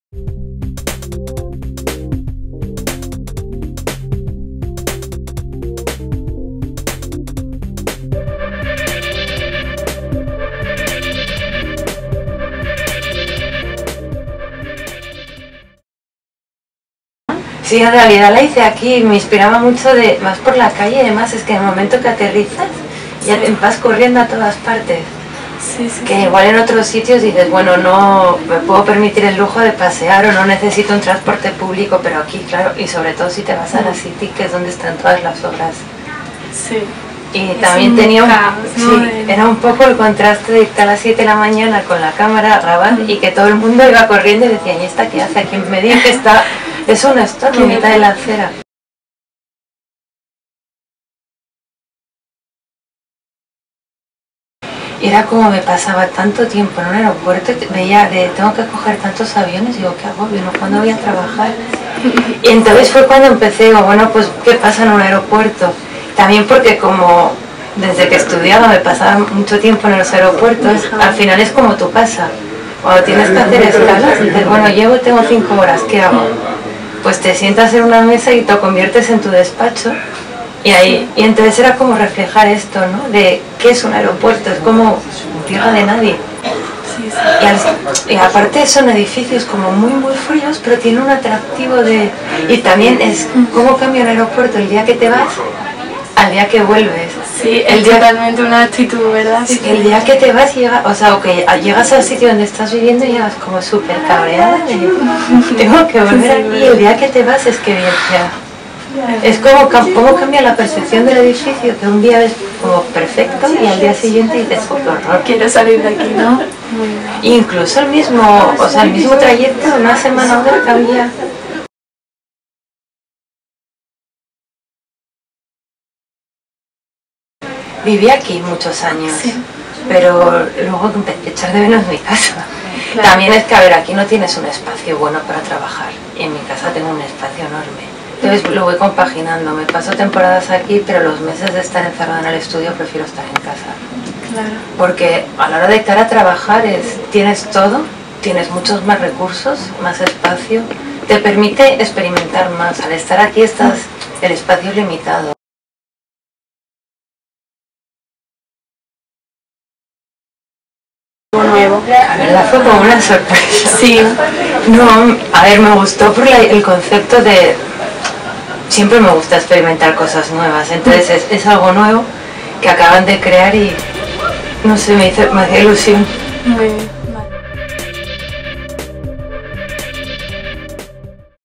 Entrevista con la artista española Susana López en Londres
El equipo de Genteventos entrevista a la artista española Susana López en Londres con motivo de la exposición ArtRooms 2015.